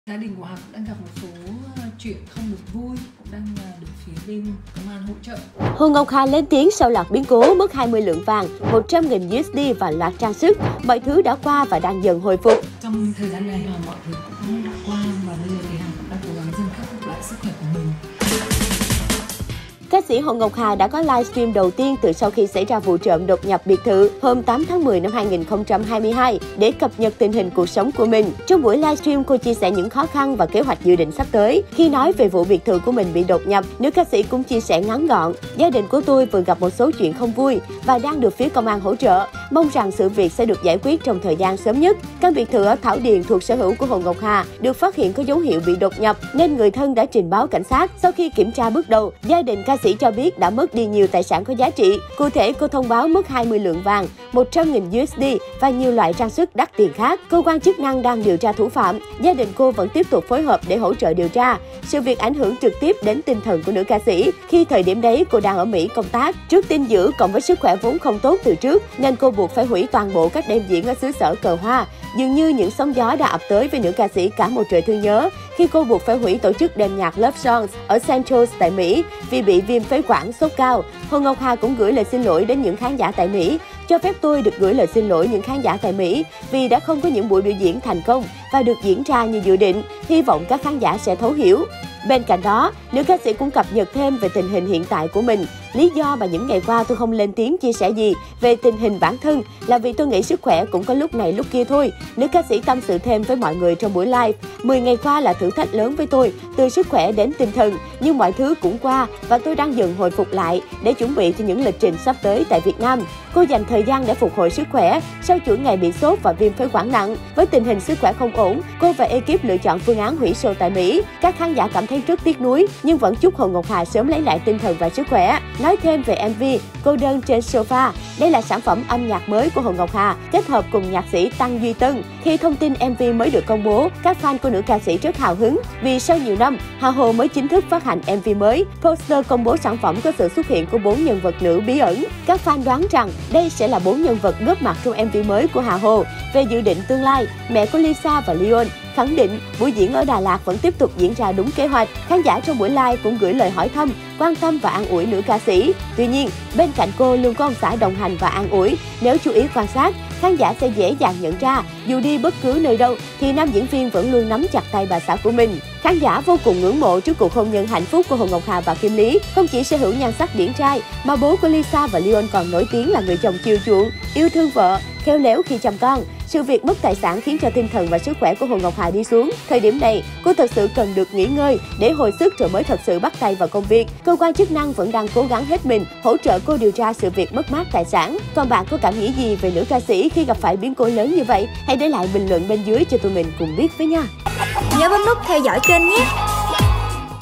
Hồ Ngọc Hà lên tiếng sau biến cố mất 20 lượng vàng, 100 nghìn USD: "Mọi thứ đang dần ổn định"
Mới đây, ca sĩ Hồ Ngọc Hà đã có livestream đầu tiên từ sau khi xảy ra vụ trộm đột nhập biệt thự hôm 8/10 để cập nhật tình hình cuộc sống hiện tại của mình. Trong buổi livestream cô chia sẻ những khó khăn trong thời điểm hiện tại và kế hoạch dự định sắp tới.

Hãy theo dõi Yan trên:
✔️Youtube Yan News :